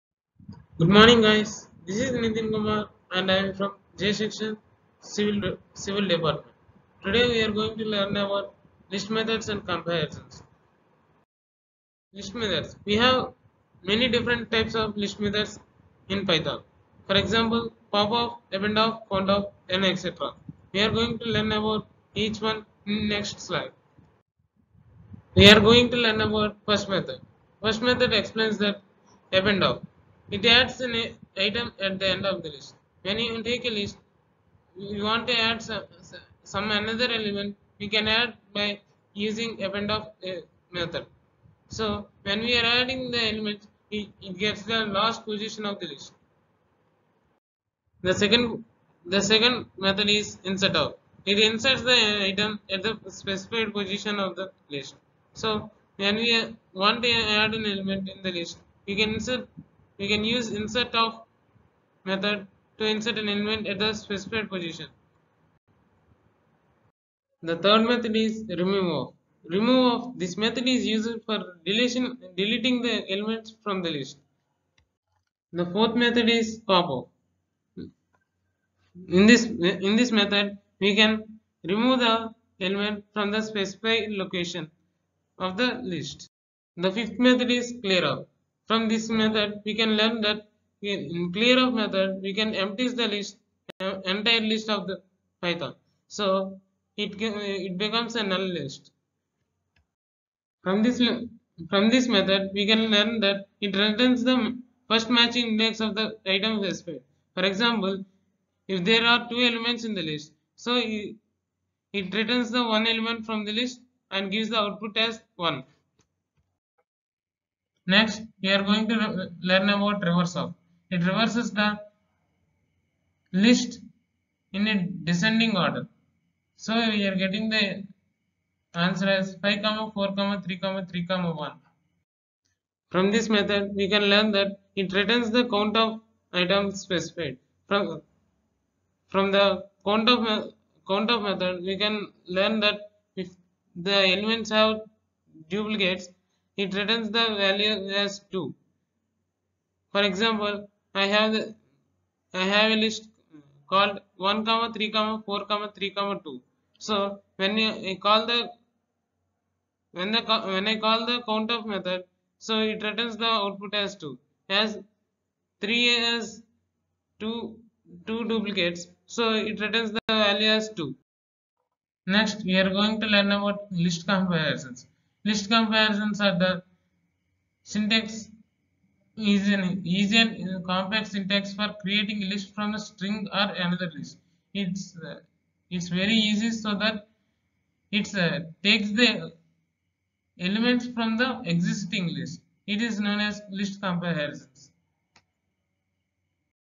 L18: List Methods
This video consists of the topic "List methods in Python" in "Unit-1" python programming
Done by "R . Nithin Kumar", Roll. No "J-20"
Note: This is friendly video notes for easy learning